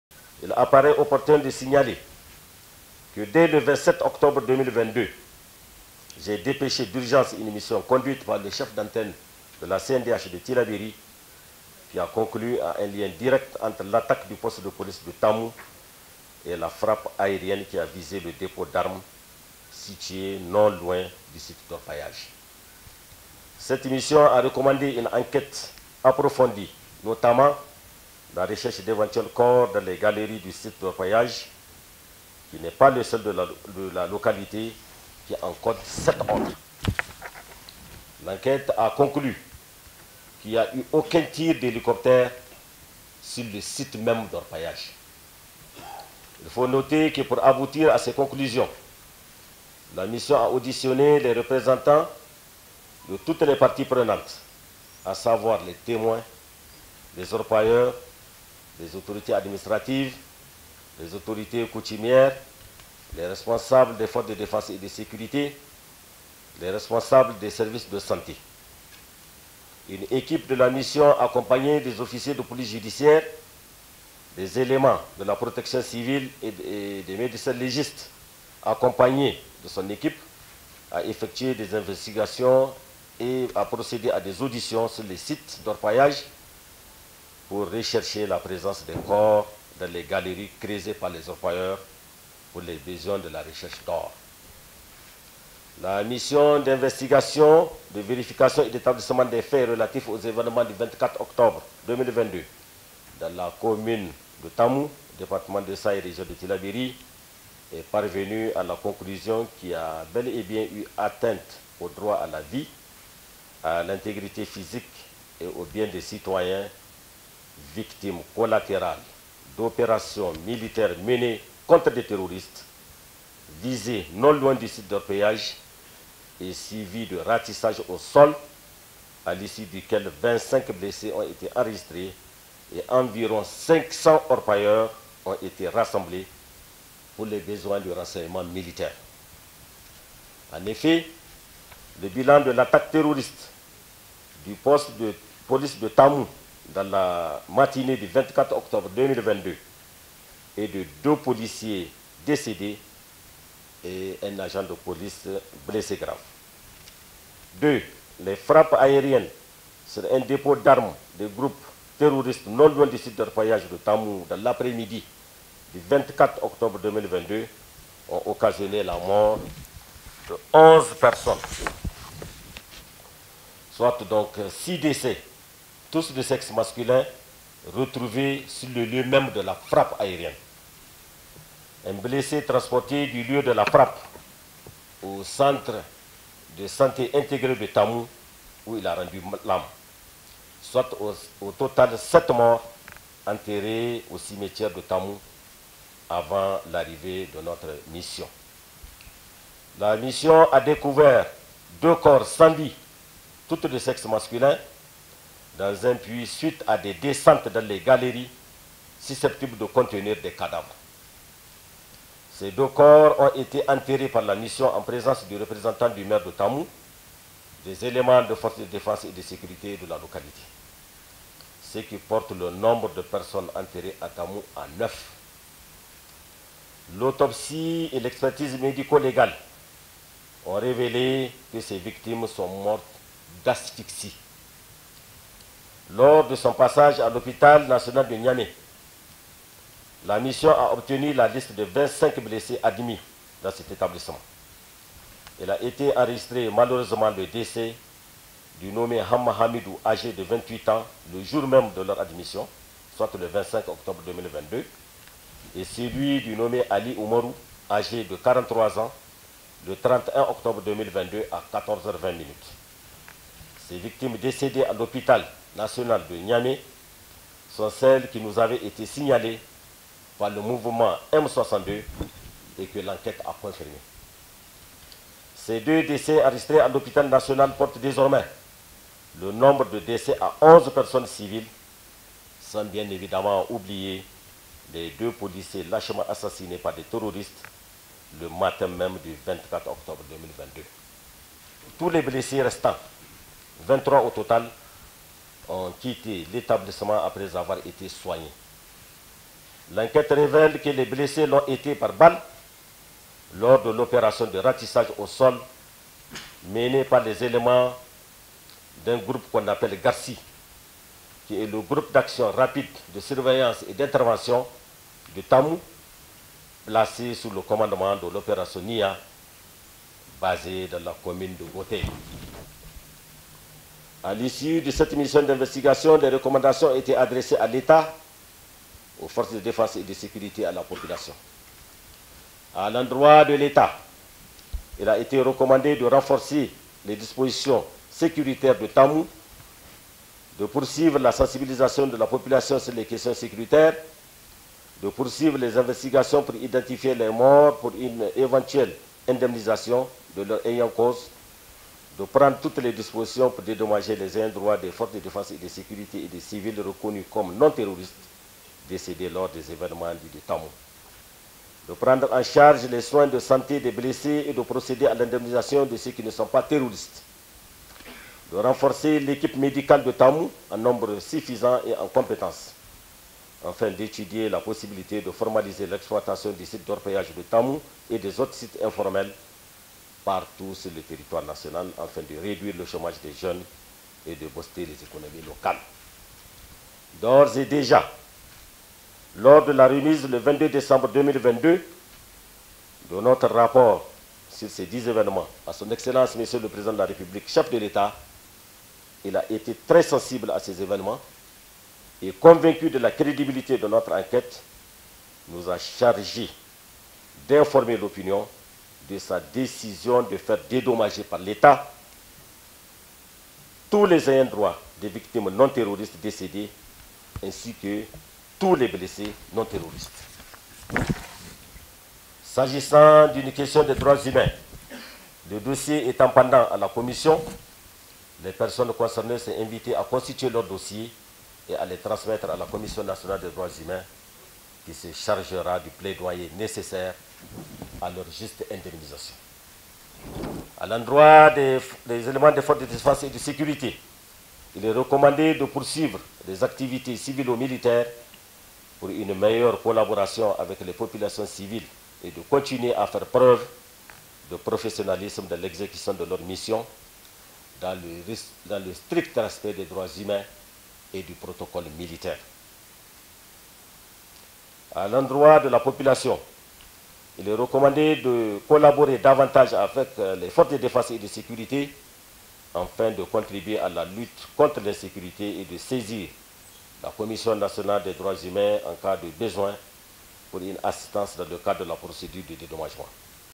NIGER : PRÉSENTATION DU RAPPORT DE LA MISSION D'INVESTIGATION, DE VÉRIFICATION ET D'ÉTABLISSEMENT...
NIGER : PRÉSENTATION DU RAPPORT DE LA MISSION D'INVESTIGATION, DE VÉRIFICATION ET D'ÉTABLISSEMENT DES FAIT RELATIFS AUX ÉVÉNEMENTS DU 24 OCTOBRE 2022 DANS LA COMMUNE RURAL DE TAMOU ( DÉPARTEMENT DE SAY)